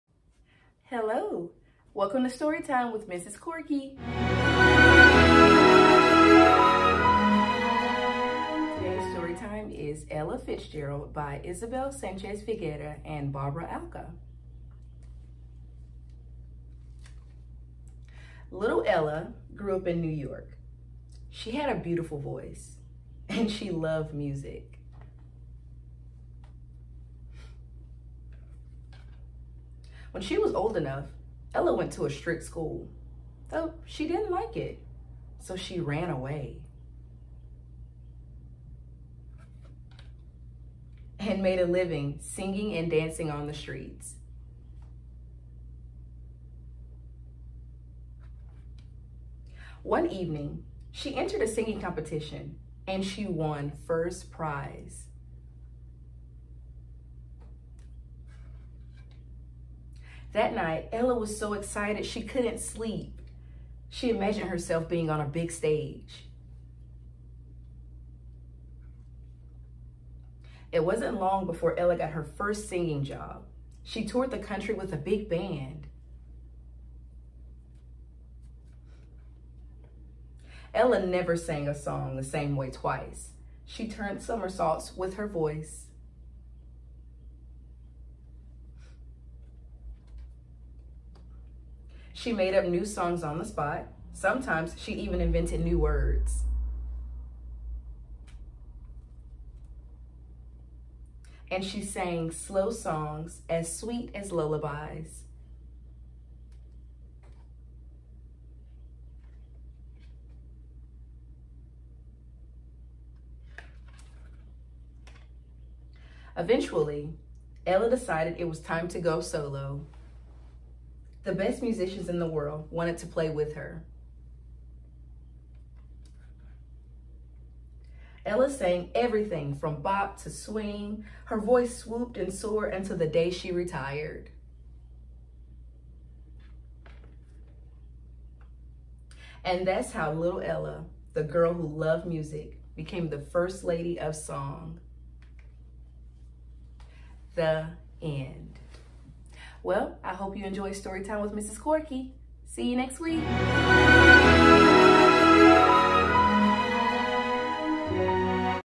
Story time with Ms. Corky! S1E1
Please enjoy your weekly story time with Ms. Corky!
Featuring fun, children’s books for read aloud!

Every Friday at 11am EST!

Produced By: Hostess City Productions

SAV Babysitting Services, LLC
Hostess City Productions, LLC
A&C Candle Company